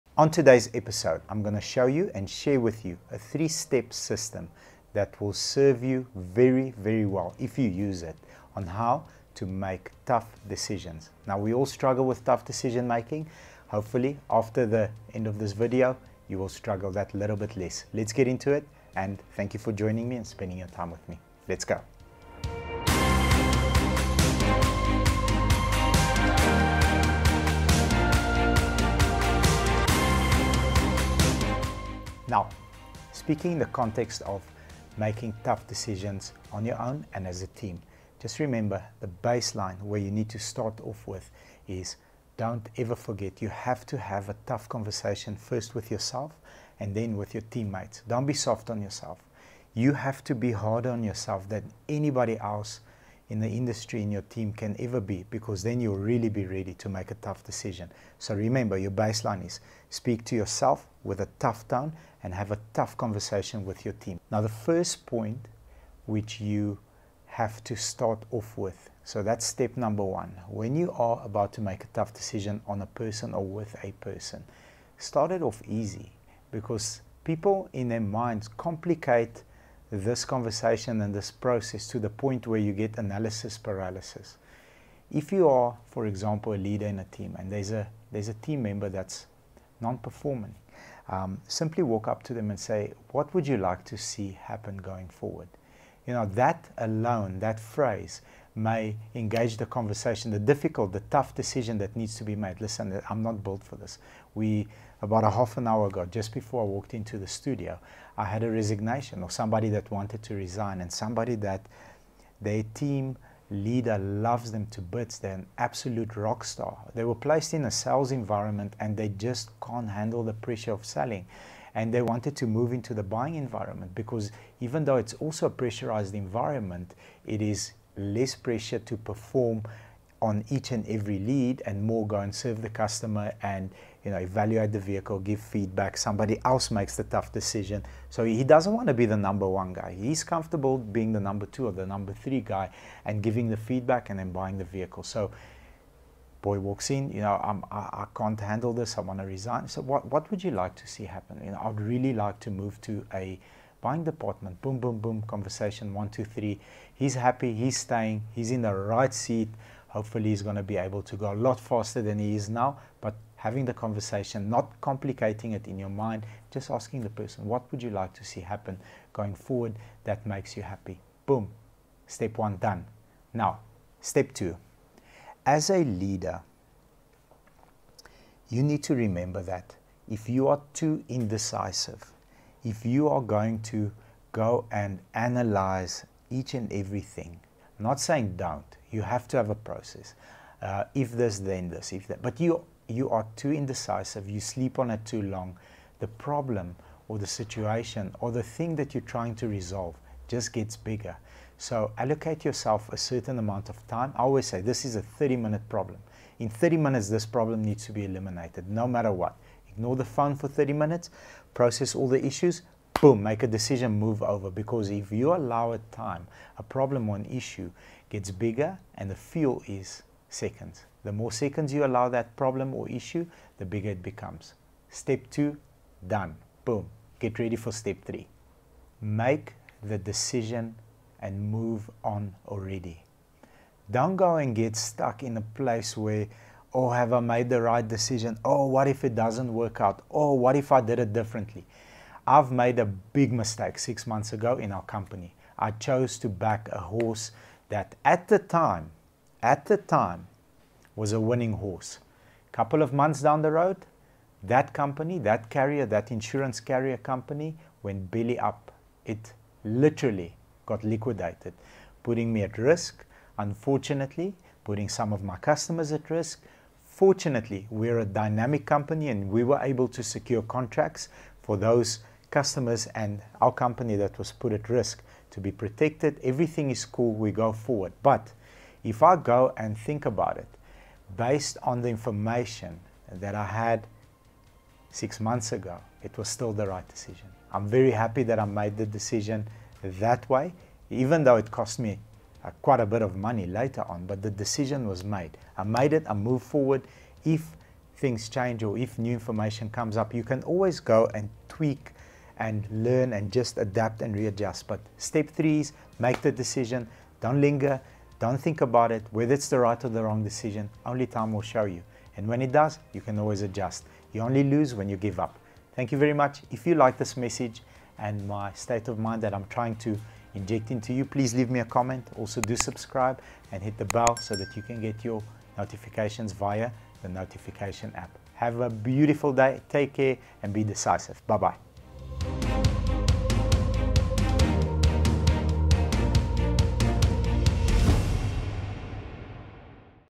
How To Make Tough Decisions In Life | Making tough choices
How to make tough decisions in life. Having to make tough decisions in life is bound to come across your path. How do you deal with it, what do you need to do in order to handle those choices effectively? Watch this episode to learn some tips and tricks to deal with those choices effectively.

0:32 The baseline
1:04 Step nr 1
2:54 Step nr 2
3:49 Step nr 3